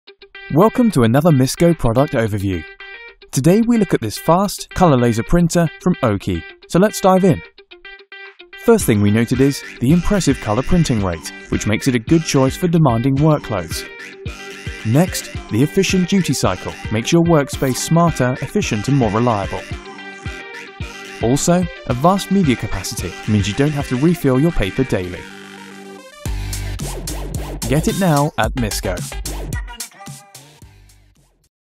OKI C822n - printer - color - LED (194109) - Misco.co.uk
This OKI C822n - Printer - color - LED - A3 - 1200 x 600 dpi - up to 23 ppm
(mono) / up to 23 ppm (color) - capacity: 400 sheets - USB, LAN is brought you